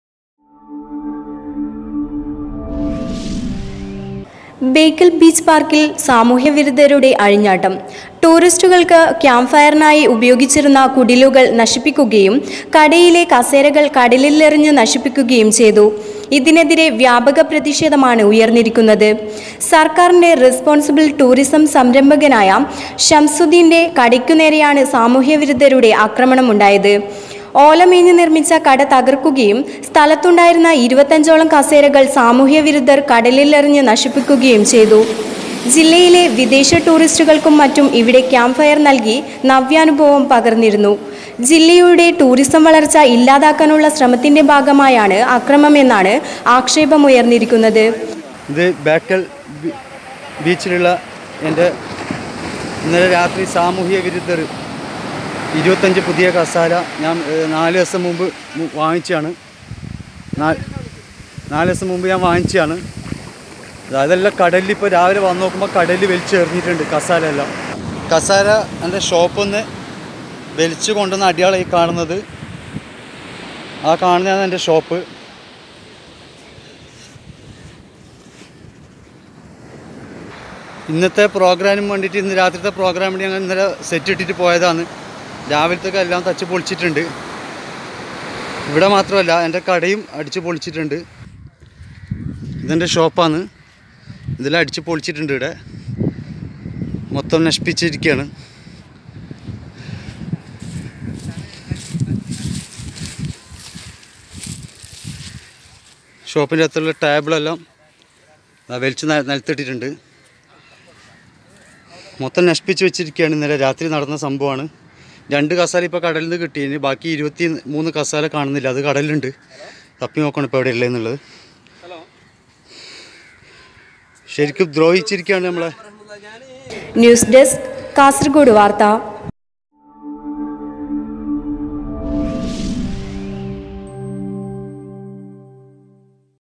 ബേക്കല്‍ ബീച്ച് പാര്‍ക്കില്‍ സാമൂഹ്യവിരുദ്ധരുടെ അഴിഞ്ഞാട്ടം
ബേക്കല്‍ ബീച്ച് പാര്‍ക്കില്‍ സാമൂഹ്യവിരുദ്ധരുടെ അഴിഞ്ഞാട്ടം; ക്യാമ്പ്ഫയറിന് ഉപയോഗിച്ചിരുന്ന കുടിലുകള്‍ നശിപ്പിച്ചു, കസേരകള്‍ കടലിലെറിഞ്ഞു, വ്യാപക പ്രതിഷേധം